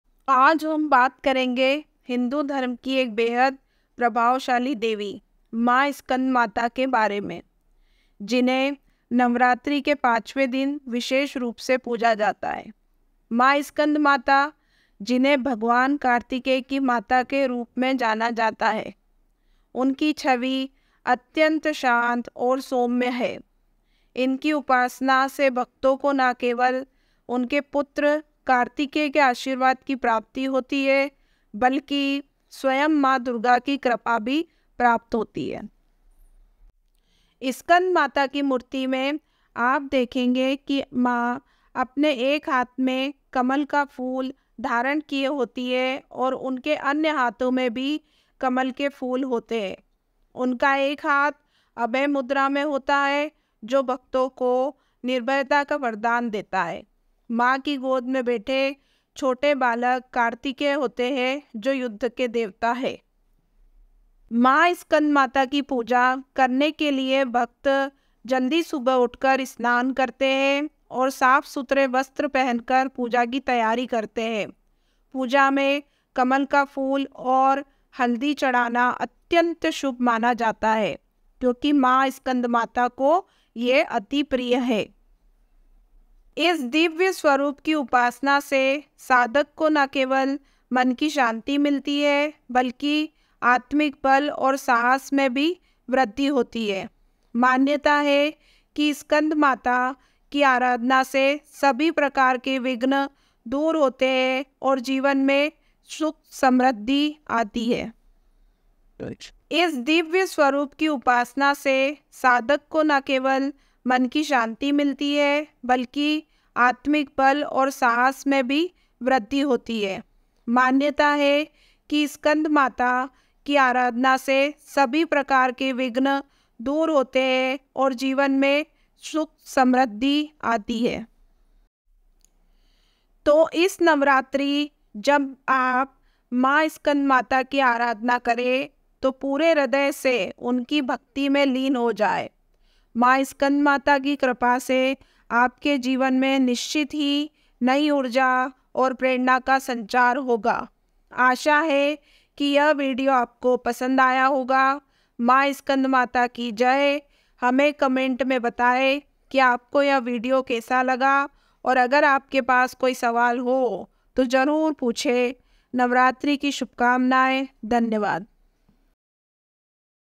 Skandmata ki katha kahani status जय माँ स्कंदमाता: नवरात्रि के पांचवें दिन की दिव्य उपासना
Explore the spiritual realm, connect with the divine, and find peace within.

नमस्कार दोस्तों! इस विशेष नवरात्रि वीडियो में, हम नवरात्रि के पांचवें दिन की पूजनीय देवी, माँ स्कंदमाता की महिमा को समर्पित करते हैं। जानिए कैसे माँ स्कंदमाता की उपासना से आपके जीवन में सकारात्मकता और शांति भर सकती है। हम बताएँगे उनकी पूजा की विधि, महत्व, और आपके जीवन पर उनके आशीर्वाद के प्रभाव के बारे में।

माँ स्कंदमाता को समर्पित इस वीडियो में, आपको मिलेगा:
- माँ स्कंदमाता का परिचय और उनकी आध्यात्मिक महत्वता।
- उनकी पूजा की सही विधि और उन्हें प्रिय सामग्री।
- माँ स्कंदमाता की कथाएं और उनसे जुड़ी अनूठी परंपराएँ।
- उनकी पूजा से मिलने वाले लाभ और आपके जीवन में उनका प्रभाव।

इस नवरात्रि, आइए हम सब मिलकर माँ स्कंदमाता की अराधना करें और उनके आशीर्वाद से अपने जीवन को धन्य बनाएं। अपने सवाल और विचार हमें कमेंट के माध्यम से बताएं। जय माँ स्कंदमाता!